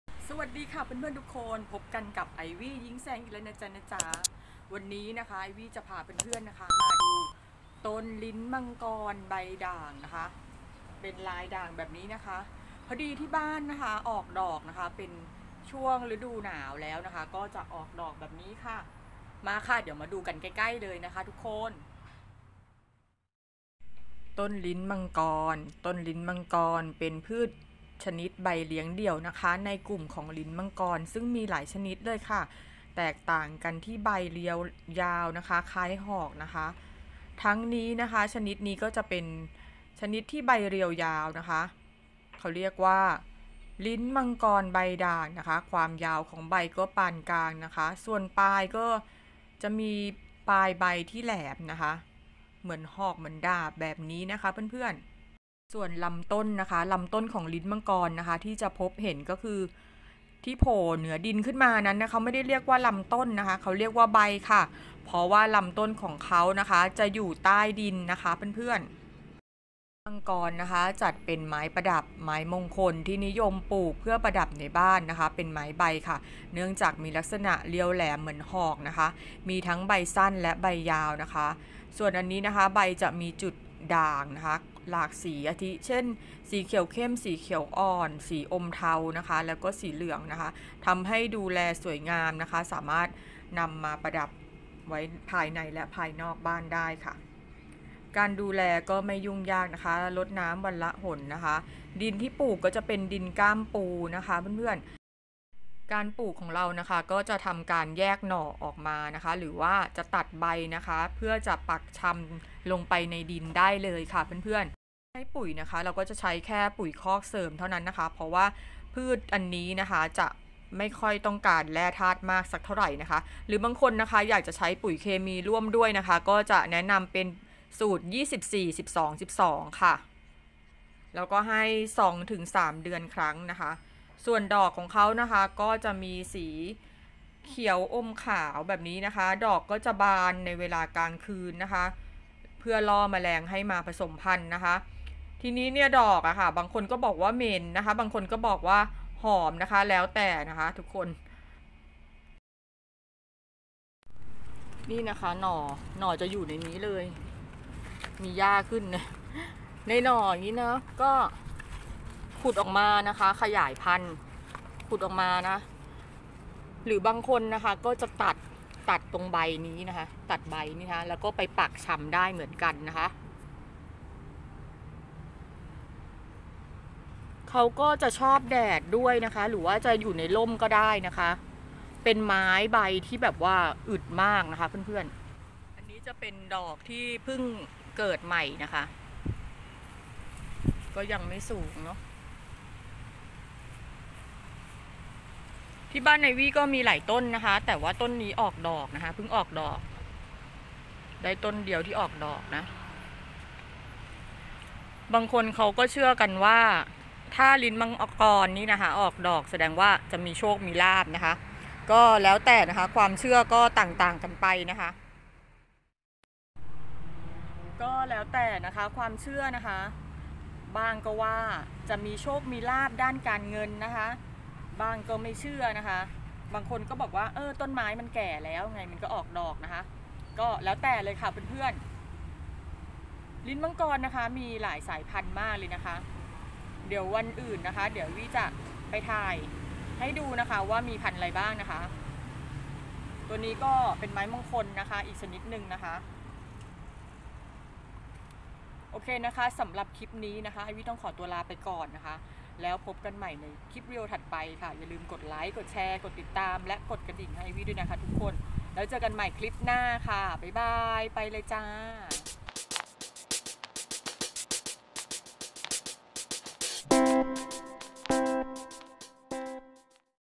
ต้นไม้มงคล ไม้ฟอกอากาศ ต้นลิ้นมังกรให้ออกดอก และวิธีดูแล | Ivy Ying-sang
ต้นลิ้นมังกร เป็นชื่อเรียกของพืชใบเลี้ยงเดี่ยวในกลุ่มของลิ้นมังกรซึ่งมีหลายชนิด แต่จะมีลักษณะเด่นที่ใบเรียวยาวคล้ายหอก ทั้งชนิดเรียวยาวมาก ยาวปานกลาง และสั้น ส่วนปลายใบมีทั้งชนิดที่ปลายใบแหลม และมีหนาม ปลายใบแหลมแต่ไม่มีหนาม และปลายใบมน นอกจากนั้น ลิ้นมังกรยังถูกนำไปเรียกพืชที่มีลักษณะใบคล้ายกัน แต่จัดอยู่ในกลุ่มของกล้วยไม้ เช่น ลิ้นมังกรใบจุด เป็นต้น รวมถึงใช้เรียกพืชบางชนิดที่จัดอยู่ในกลุ่มของกระบองเพชรลิ้นมังกรนิยมปลูกทั้งในแปลงจัดสวน และการปลูกในกระถาง ซึ่งนิยมขยายพันธุ์ด้วย 2 วิธี คือ การขยายพันธุ์แบบปักชำใบ และการขยายพันธุ์แบบแยกเหง้าปลูก แต่ที่นิยมที่สุดจะเป็นการปักชำใบ เนื่องจาก สะดวก รวดเร็ว และขยายพันธุ์ได้ครั้งละจำนวนมาก

การปลูกในแปลงหรือปลูกในกระถางนั้น สามารถปลูกได้ทั้งในที่ร่ม และที่กลางแจ้ง เพราะลิ้นมังกรเป็นพืชที่เติบโตได้ดีทั้งที่อยู่ในแสงน้อย และกลางแจ้ง แต่ลิ้นมังกรบางชนิดที่มีลักษณะอวบน้ำนั้น มักจะไม่ชอบแสงแดดจัด เพราะน้ำในใบที่มีมากจะร้อนทำให้เกิดอาการใบลวกได้ง่าย
ลิ้นมังกร จัดเป็นไม้ใบประดับ และไม้มงคล ที่นิยมเพื่อการประดับใบ เนื่องจาก ใบมีลักษณะเรียวยาวคล้ายหอก มีทั้งใบสั้น และใบยาว ใบมีจุดด่างของหลายสีอาทิ สีเขียวเข้ม สีเขียวอ่อน สีเขียวอมเทา และสีเหลือง ทำให้แลดูสวยงาม และเป็นที่แปลกตาเมื่อพบเห็นการเตรียมดิน
การเตรียมดินสำหรับการปลูกในแปลง มักไม่มีขั้นตอนยุ่งยาก เนื่องจาก จะใช้ดินในแปลงเป็นจุดปลูก แต่จะต้องทำการกำจัดวัชพืช และพรวนด้วย พร้อมกับใส่ปุ๋ยคอกคลุกผสมด้วย ส่วนการปลูกในกระถางซึ่งจะเป็นวิธีที่นิยมมาก จำเป็นต้องเตรียมวัสดุปลูก ด้วยการผสมดิน และทรายร่วมกับวัสดุอินทรีย์อื่นๆ อาทิ แกลบดำ ขุยมะพร้าว เป็นต้น อัตราส่วนผสมดิน:ทราย และวัสดุปลูกที่ 1 : 0.5 : 2 แล้วคลุกผสมให้เข้ากัน ก่อนจะบรรจุใส่กระถาง

วิธีปลูก
– การปลูกด้วยการปักชำใบจะใช้ใบจากต้นแม่ด้วยการตัดใบให้ชิดโคนใบบริเวณหน้าดิน หลังจากนั้นแบ่งใบออกเป็นส่วนๆ ยาวประมาณ 10-15 ซม. และเฉือนโคนท่อนใบให้เฉียงประมาณ 45 องศา แล้วค่อยนำปักเสียบลงในกระถางลึกประมาณ 5-10 ซม. กระถางละ 1-3 ใบ ส่วนการปลูกในแปลง อาจใช้วิธีปลูกปักชำโดยตรง หรือ นำใบที่ปักชำในกระถางที่ติดรากแล้วนำลงปลูกในแปลงอีกครั้ง
– ส่วนการปลูกด้วยการแยกเหง้า จะทำด้วยการขุดเหง้าลิ้นมังกร แล้วตัดแบ่งเหง้าออกเป็นส่วนๆ โดยควรให้เหง้ามีใบติดมาด้วย 1-2 ใบ และให้ตัดเฉือนใบส่วนปลายทิ้งเป็นรูปปากฉลามให้เหลือใบยาวประมาณ 20 ซม. ส่วนใบที่ตัดทิ้งให้นำมาปักชำต่อได้
การดูแลลิ้นมังกร
หลังจากการปลูก ให้รดน้ำทุกวัน วันละ 1 ครั้ง พอให้หน้าดินชุ่ม และให้รดน้ำต่อเนื่องจนใบแตกหน่อใหม่แล้ว และหลังจากที่หน่อ และใบใหม่ยาวได้มากกว่า 5-10 ซม. ค่อยลดปริมาณน้ำที่ให้ลง อาจให้เพียง 2-3 ครั้ง/สัปดาห์ ขึ้นอยู่กับสภาพความชุ่มของหน้าดิน
ส่วนการใส่ปุ๋ยนั้น ไม่จำเป็นต้องใส่มาก เพราะพืชชนิดนี้ไม่ต้องการธาตุอาหารมาก เพียงให้ปุ๋ยคอกเสริมเป็นประจำทุกๆ 2-3 เดือน ก็เพียงพอแล้ว และอาจให้ปุ๋ยเคมีร่วม เช่น สูตร 24-12-12 เพื่อเร่งการเติบโตของใบร่วมด้วย
1. เนื่องจากใบลิ้นมังกรเรียวยาวคล้ายดาบหรือหอก ใบมีจุดเป็นด่างประทั่วใบ ทำให้แลดูสวยงาม และแปลกตาผู้ที่พบเห็นจนกลายเป็นไม้ประดับใบที่นิยมปลูกชนิดหนึ่ง
2. ลิ้นมังกรนิยมปลูกเป็นไม้มงคล เนื่องจากลักษณะใบของลิ้นมังกรที่มีรูปร่างคล้ายหอก ทำให้มีความเชื่อว่า หอกนี้ เป็นหอกของพระอินทร์หรือเทวดาที่จะช่วยปกป้องภัยอันตรายต่างๆไม่ให้กล้ำกายเข้าใกล้คนในครอบครัว
สรรพคุณลิ้นมังกร
ใบหรือเหง้าของลิ้นมังกรยังไม่มีการศึกษาสรรพคุณที่แน่ชัด แต่ใบของลิ้นมังกรเกือบทุกชนิดมีรสขม ซึ่งจากการรวบรวมเอกสาร และการนำเสนอในเว็บไ๙ต์ต่าง พบว่า ใบลิ้นมังกรมีสรรพคุณ ดังนี้
– ทำให้ชุ่มคอ แก้ไอ และขับเสมหะ
– ลดอาการไข้ และเป็นหวัด
– ใบนำมาบดใช้ประคบแผลกัดต่อยของแมลง ช่วยลดอาการปวด
– ใบนำมาบดใช้ประคบห้ามเลือดจากบาดแผล
– ใบใช้บดหรือบี้ทารักแผล ทำให้แผลหายเร็ว
 ลิ้นมังกรขอบเหลืองใบสั้น (ใบแบนสั้น กลางใบเขียว ขอบสีเหลือง)
– ลิ้นมังกรลายด่าง/ลิ้นมังกรพื้นเมือง (ใบแบนยาวมีด่างทั่วใบ ใบไม่มีขอบสี)
– ลิ้นมังกรลายด่างแคระ (ใบแบนสั้นมีด่างทั่วใบ ใบไม่มีขอบสี)
– ลิ้นมังกรใบกลม (ใบกลม เรียวยาว )
– ลิ้นมังกรเขี้ยวหมู (ใบแบนสั้น ใบด่างขอบใบไม่มีสี)
– ลิ้นมังกรเจดีย์หยก (ใบแบนสั้น ใบสีเขียวเข้ม ไม่มีลายประ แตกใบเรียงถี่ขึ้นสูงจนมีรูปทรงเป็นเจดีย์)
– ลิ้นมังกรครีบปลาวาฬด่าง (ใบแบนสั้น แผนใบกว้าง ปลายใบแหลม มีลายสีเขียวแกมสีเหลืองในแนวตั้งของใบ)
– ลิ้นมังกรใบจุด (ใบแบนสัน ปลายใบมน มีจุดสีขาวทั่วใบ จัดอยู่ในกลุ่มของกล้วยไม้ )

ID Line 🎈Ivyyingsang